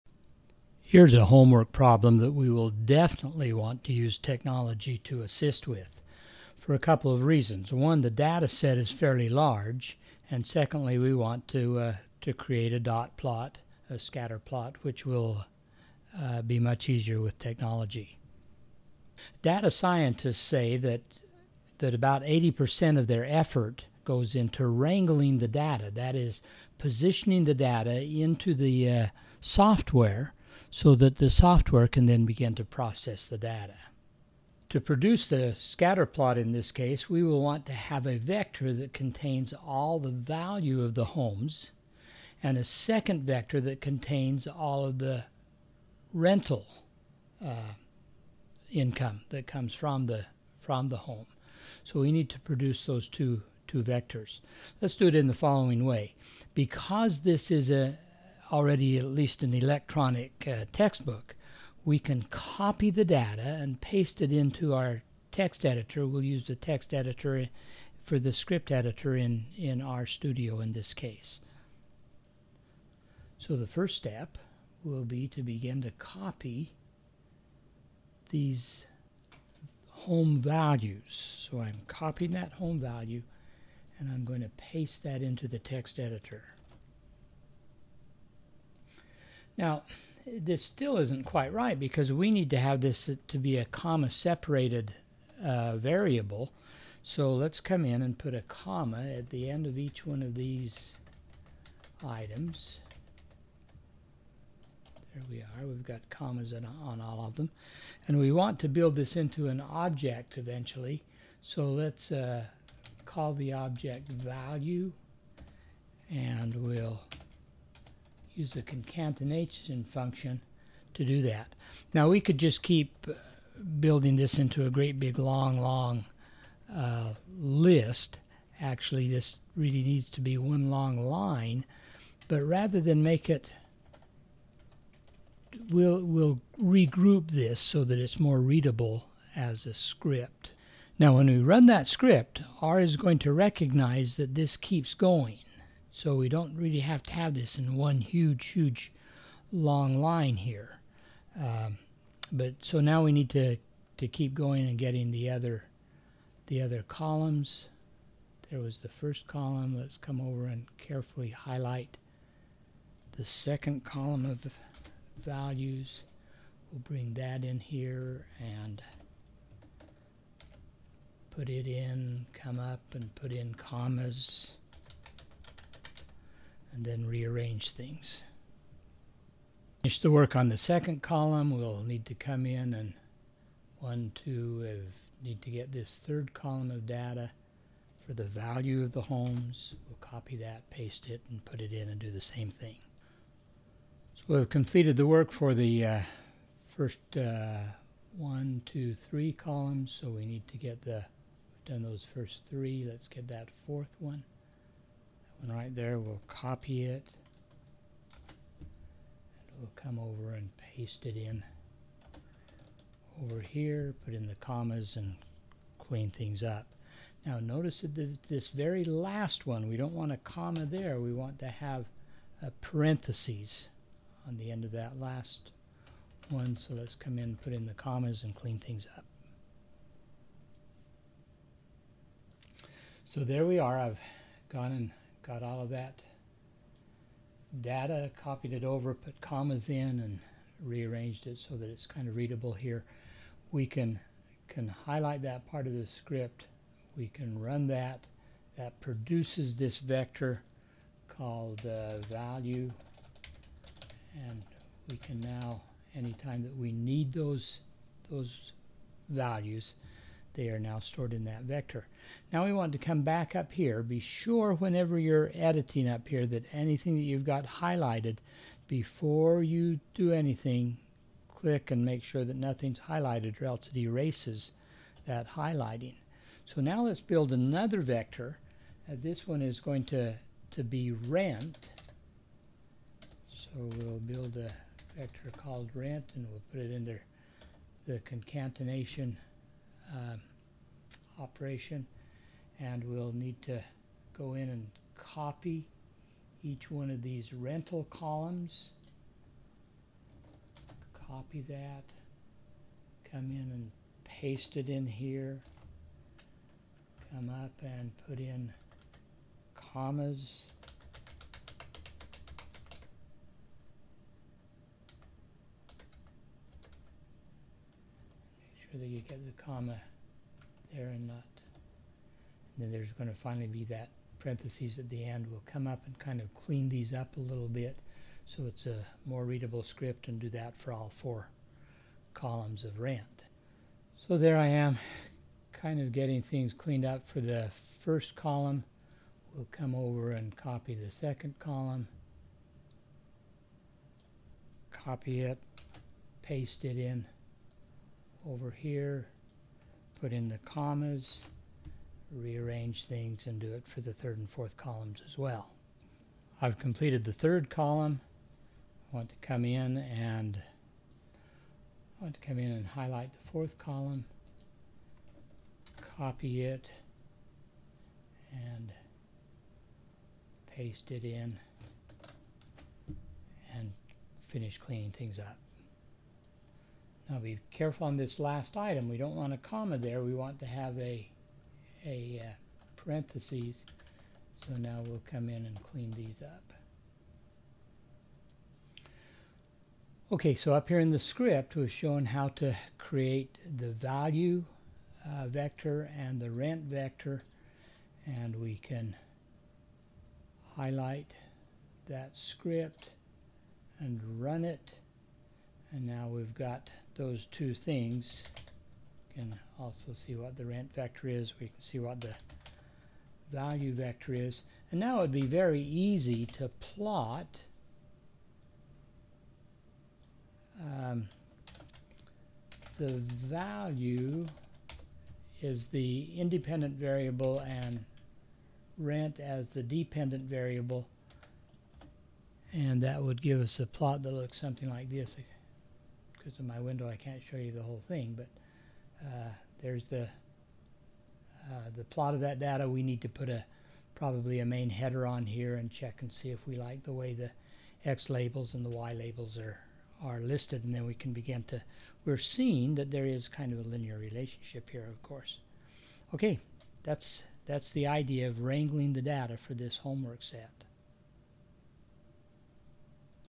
WranglingDataHomeworkProb4
Brute force wrangling of data for a homework problem. A note for students in an elementary statistics class.